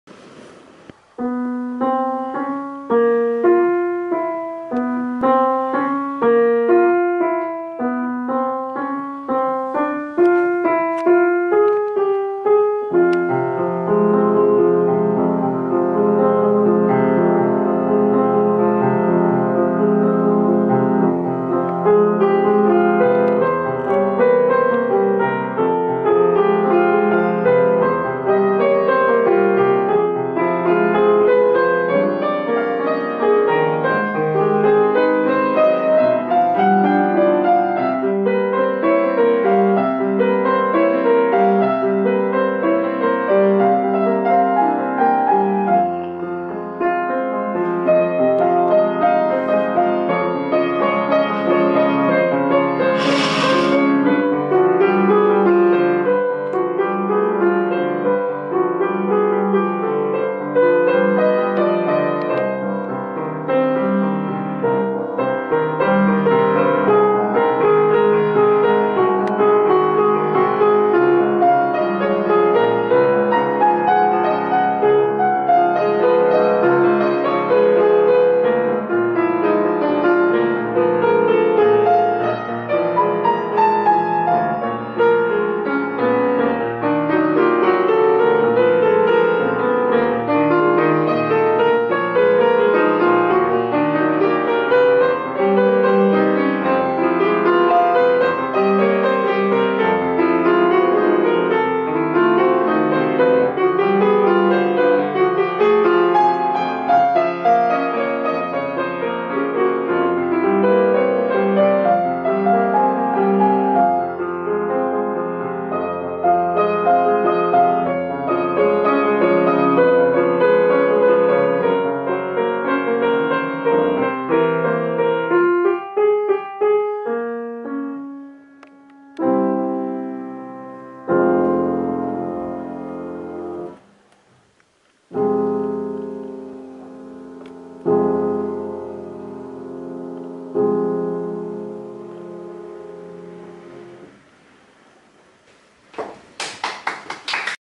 CAFE DES AMATEURS le 22 avril 2017 , Paris
Lada Bell
Chopin Etude

Nous invitons des musiciens amateurs de Paris à participer à nos concerts mensuels
FACEBOOK - CAFE DES AMATEURS, le club des musiciens amateurs de musique classique de Paris